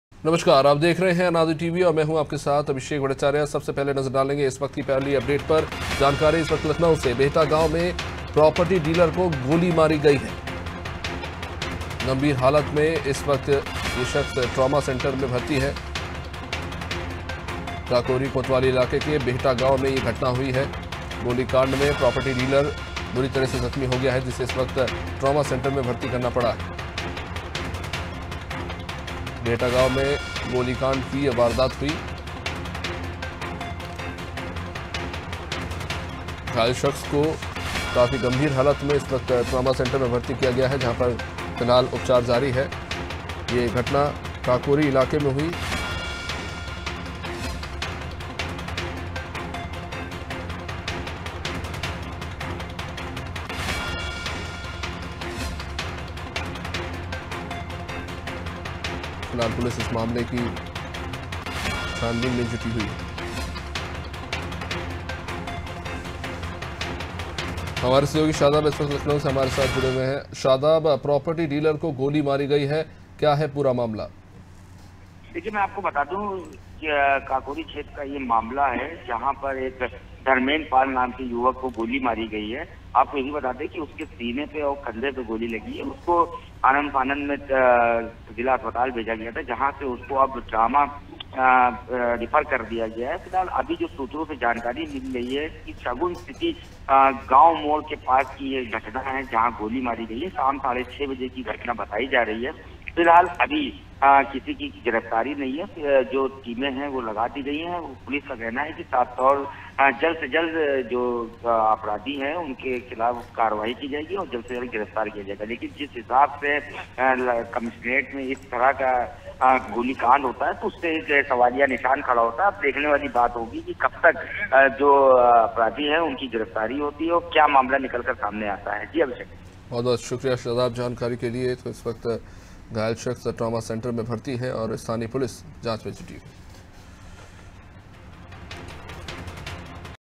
Lucknow : बेहटा गांव में प्रॉपर्टी डीलर को मारी गोली | UP BREAKING NEWS
Lucknow : बेहटा गांव में प्रॉपर्टी डीलर को मारी गोली |